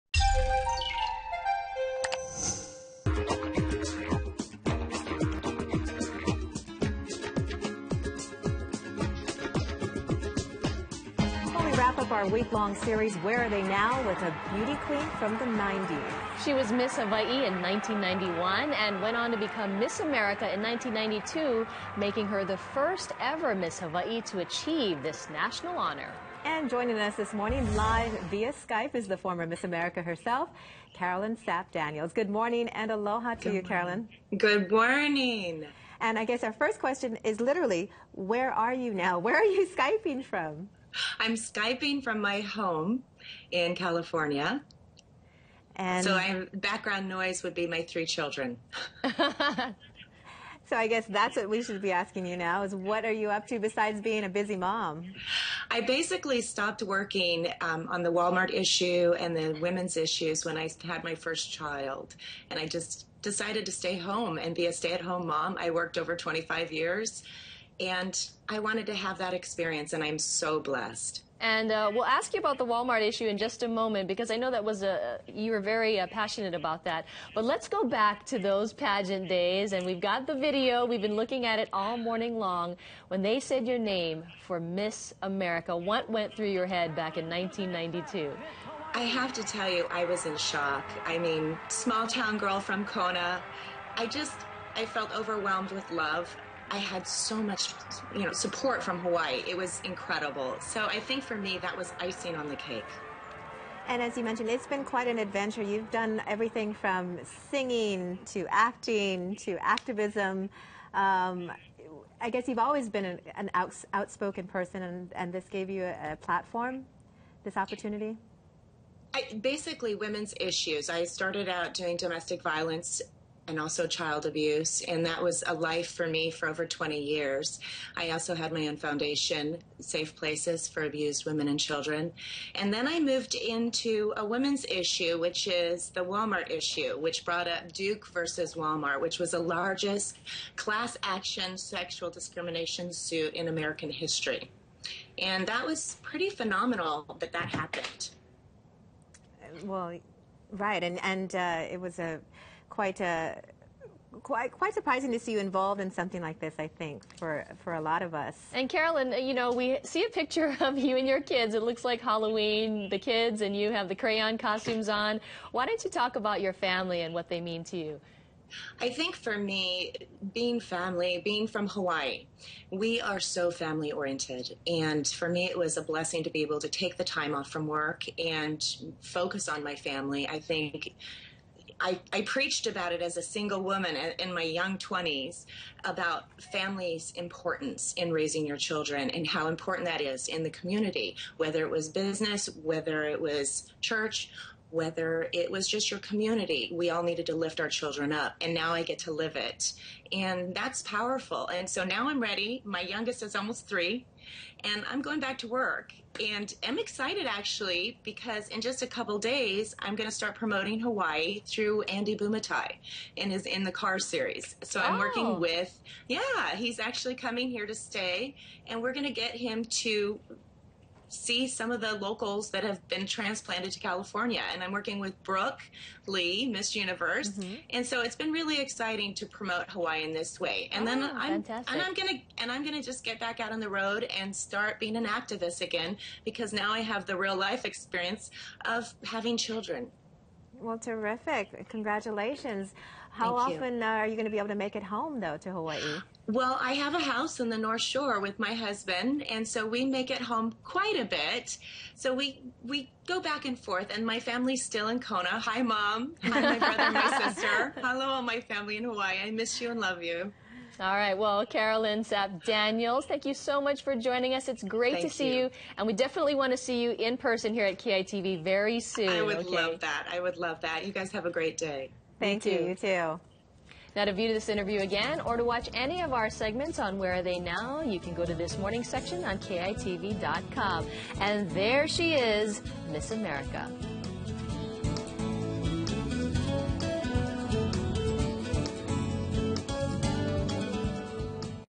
Where Are They Now: Carolyn Sapp
KITV4 talks to Carolyn Sapp, who was Miss Hawaii 1991 and became Miss America in 1992. For news you need to know,